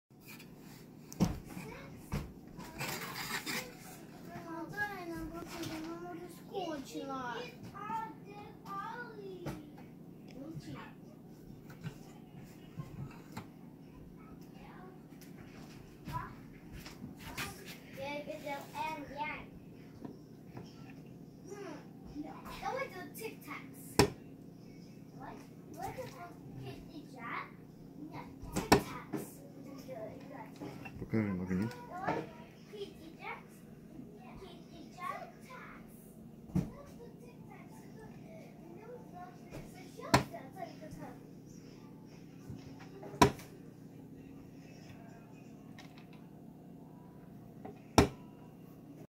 Rustic mailbox. Full functionality demonstration
Here is one of the mailboxes which creates our workshop. Posting this short video to show you the quality and how all mailboxes' mechanism works. The door holding on the chain and closing on the heavy duty magnets. Easy to open and close, but magnets are strong to hold it. Flag moves easily, too, and keeps strong on the top and bottom by the hooks. Mailbox made from the new wood, hand carved and finished with the natural wood finish. Cedar door, pine sides. New metal for the roof and numbers, hand forged. Unique and high quality artwork! , Etsy, Custommade stores